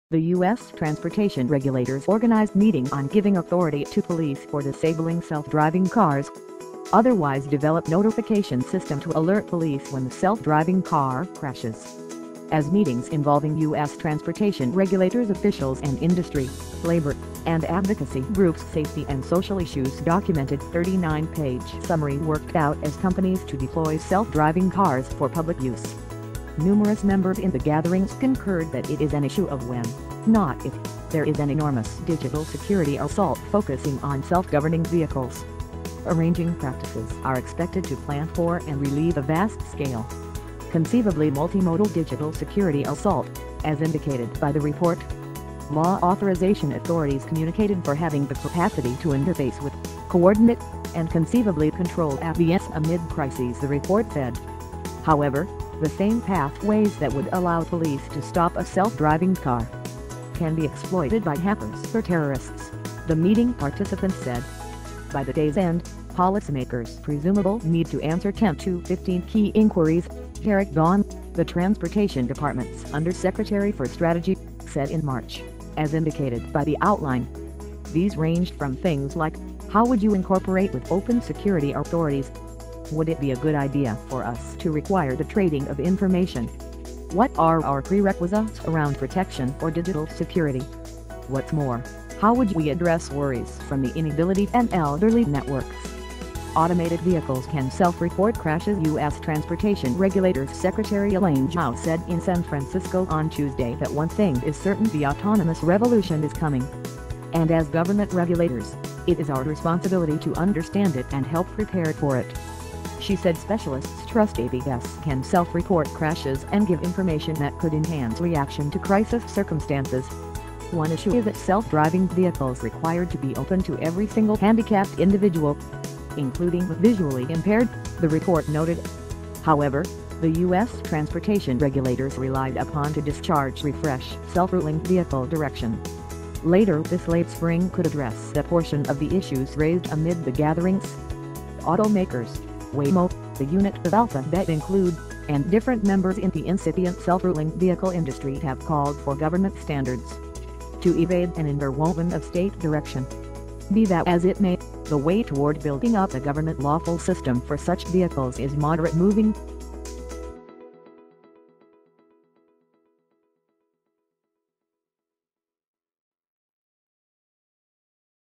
U.S. transportation regulators struggle with self-driving vehicle security
The U.S. transportation regulators organized meeting on giving authority to police for disabling self-driving cars.